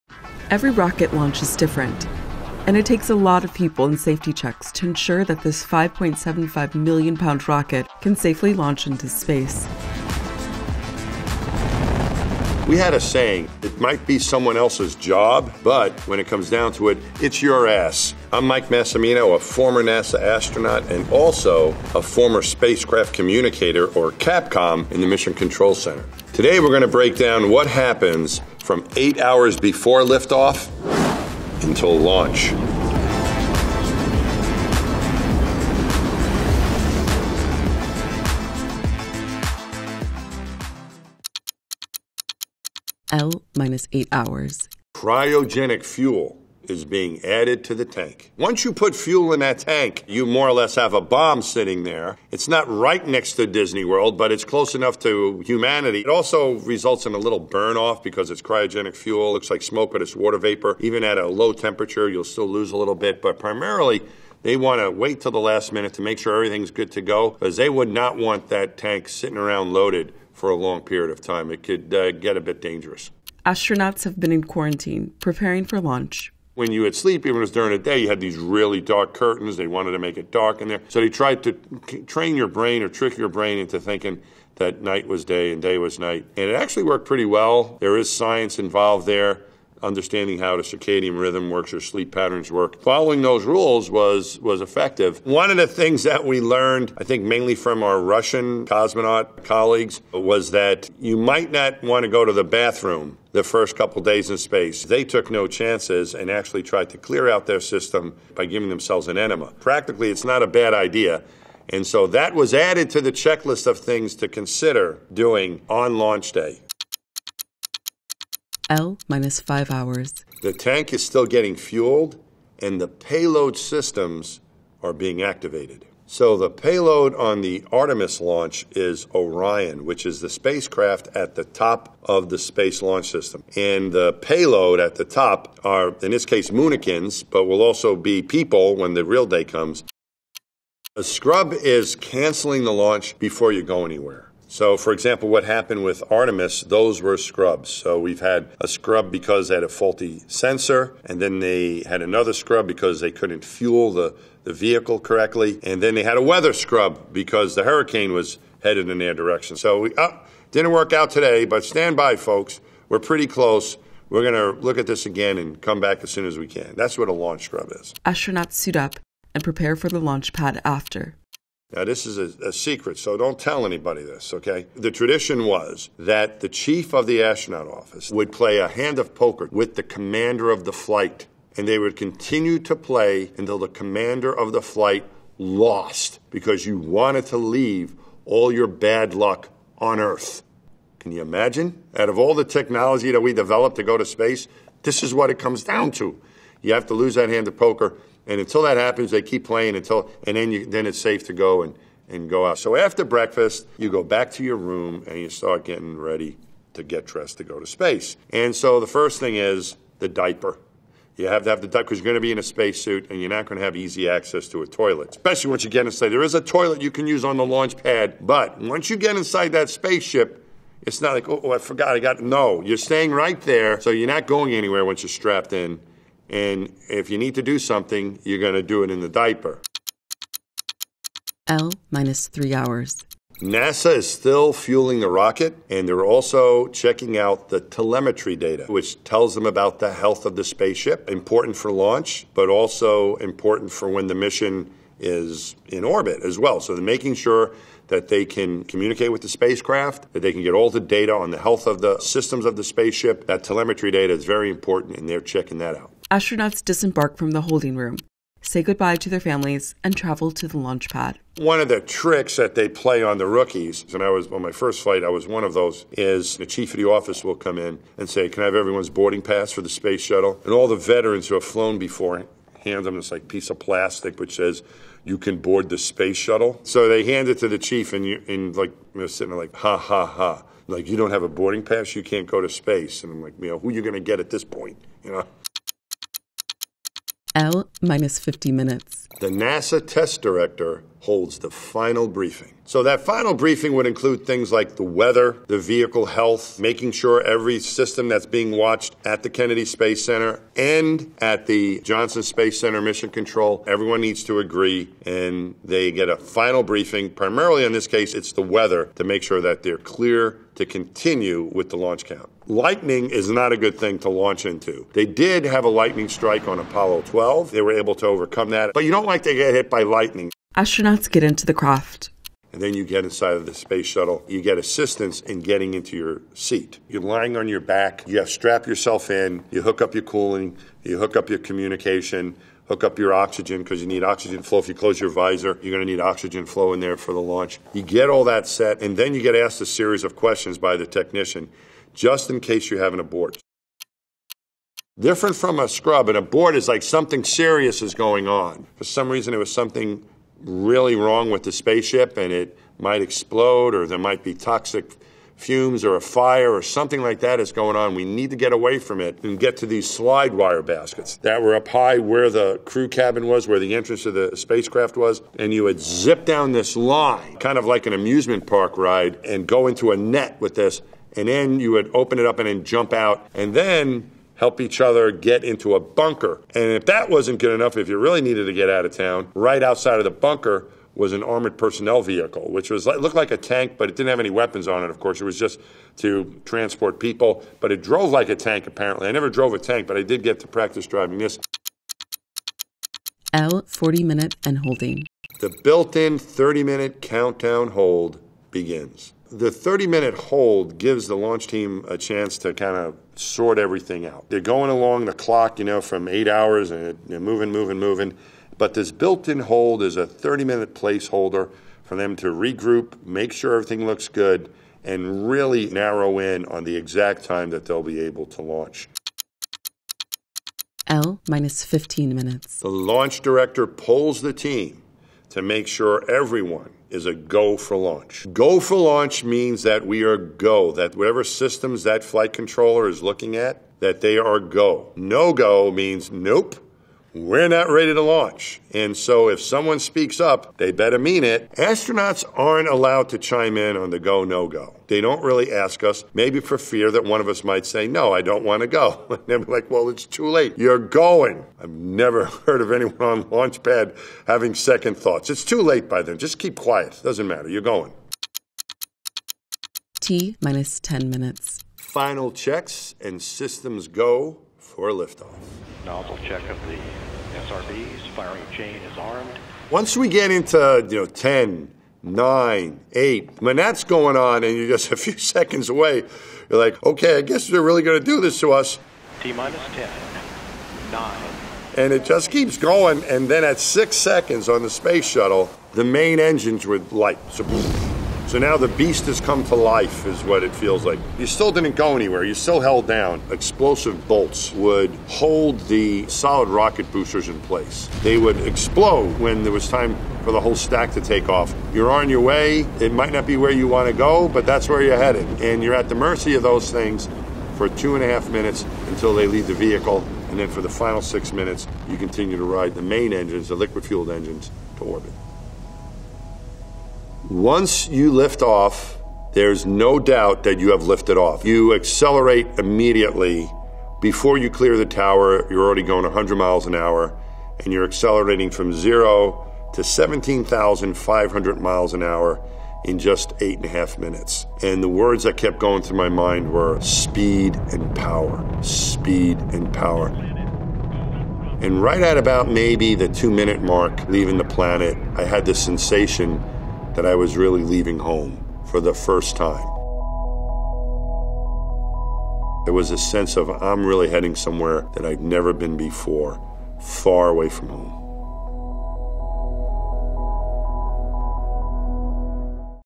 火箭發射前8小時NASA太空人都在做什麼？ Every Step Before A Successful Rocket Launch｜科普長知識｜GQ Taiwan
科普系列持續更新！訂閱GQ準時不錯過
每一次火箭發射都是獨一無二的，需要大量人員和安全檢查才能確保一枚 575 萬磅的火箭能夠安全發射到太空。 前 NASA 太空人麥克·馬西米諾（Mike Massimino）解釋了從升空前 8 小時到發射之間發生的所有事情。

譯者：Dutch 徐定遠